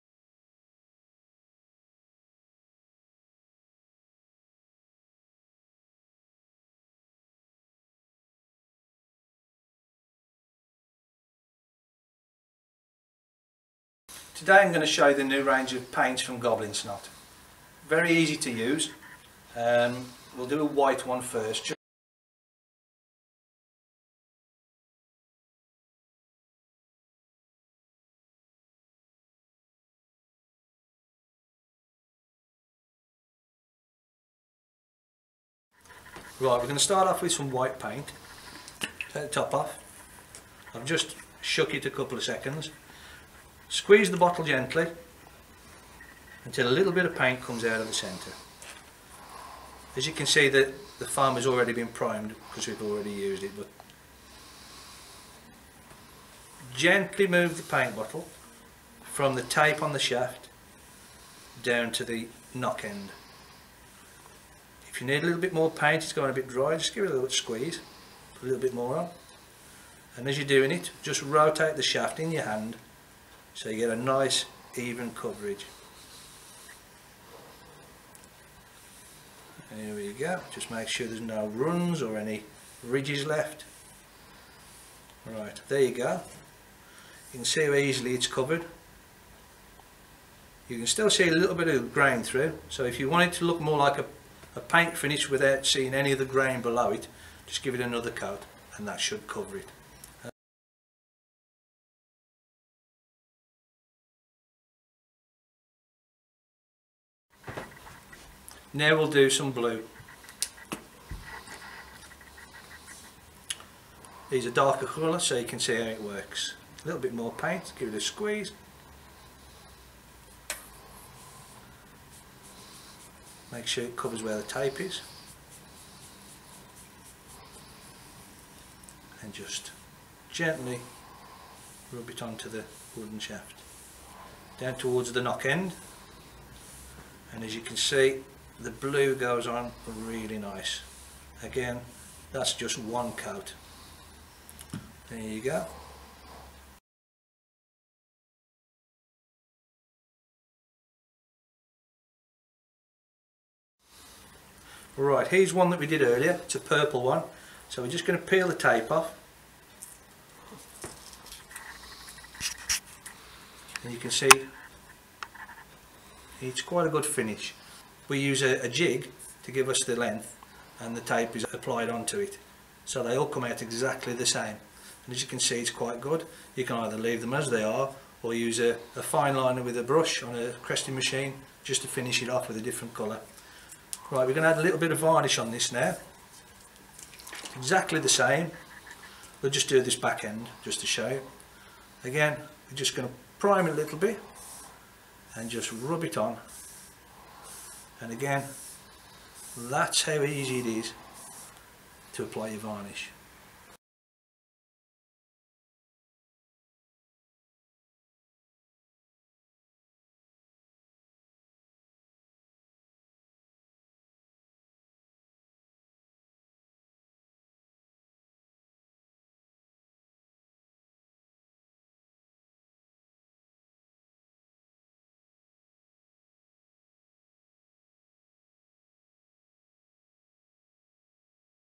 Goblin Snot Paint System
The alternative to expensive dipping tubes for arrow painting.
Save time and money with the New Goblin Snot Arrow Painting System